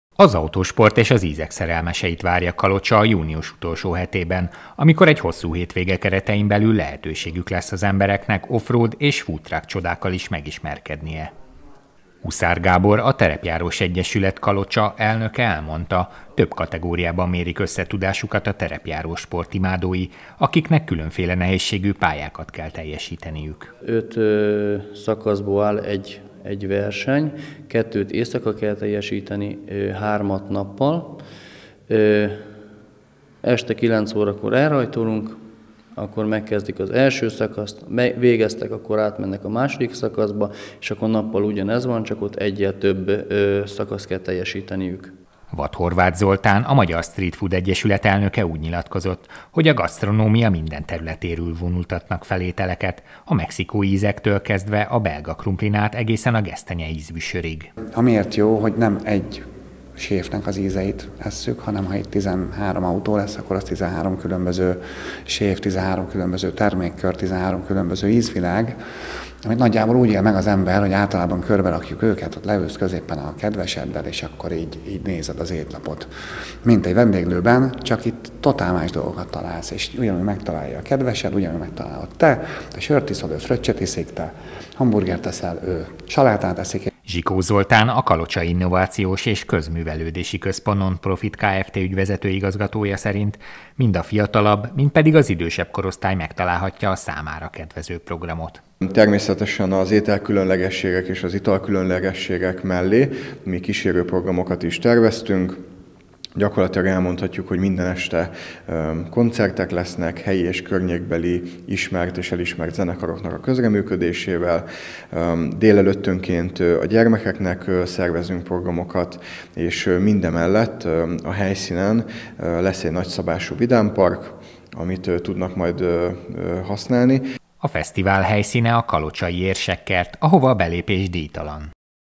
Off Road és Food Truck show - három napos programsorozat Kalocsán
Három napos rendezvénysorozatnak ad otthont Kalocsa. Június 24-e és Június 26-a között kerül megrendezésre a városban a Bács-Trophy Off Road bajnokság harmadik futama, de ezen kívül egy Food Truck shownak valamint gyerekprogramoknak is részesei lehetnek a kilátogatók.